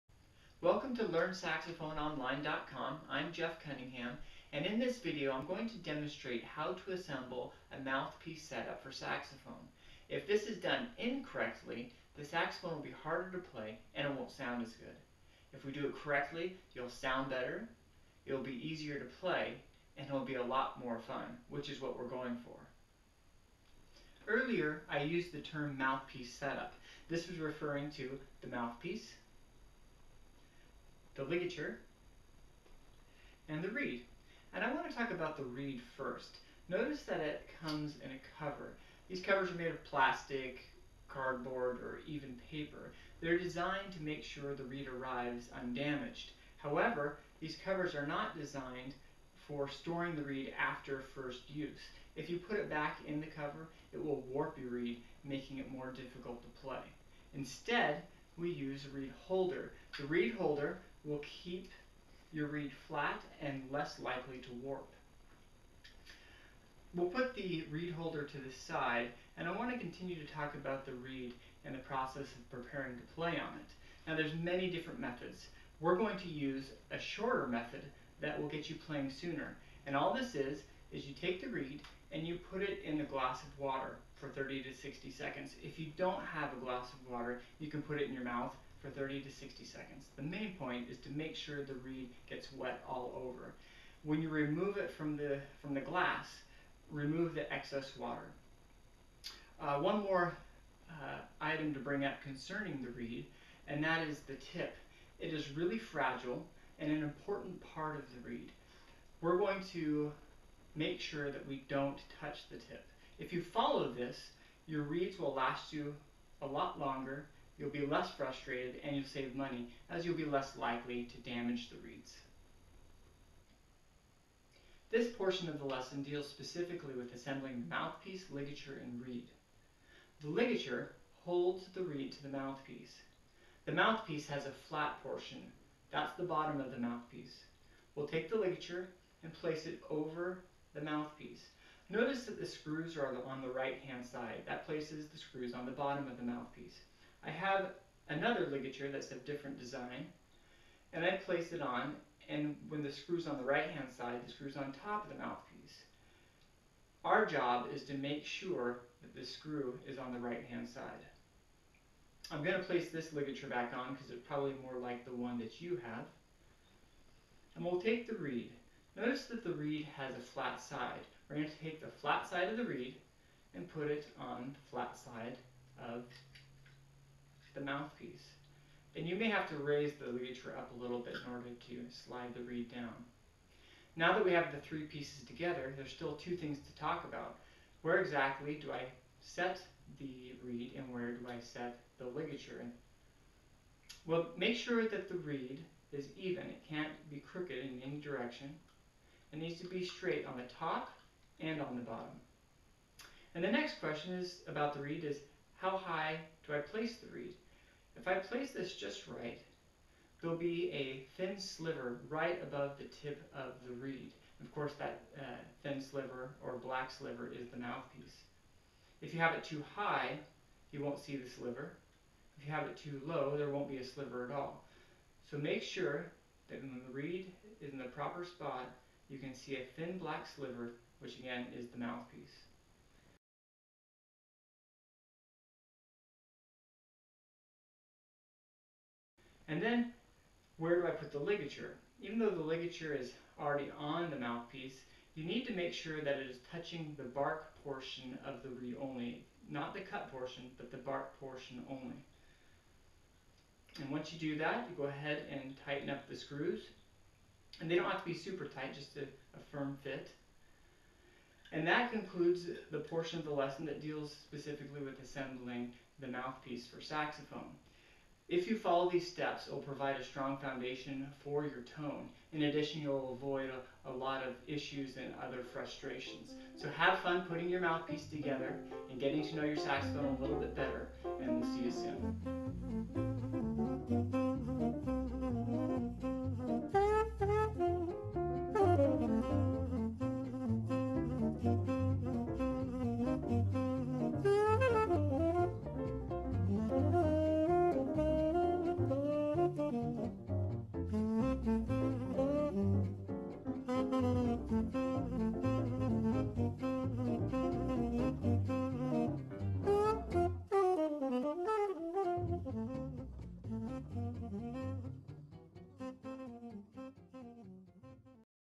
Saxophone Mouthpiece Assembly
Just fill out the form in the middle of the page.

How to assemble a saxophone mouthpiece setup. Learn how to put the reed, ligature, and mouthpiece together correctly. This saxophone lesson is an important step when learning how to play saxophone.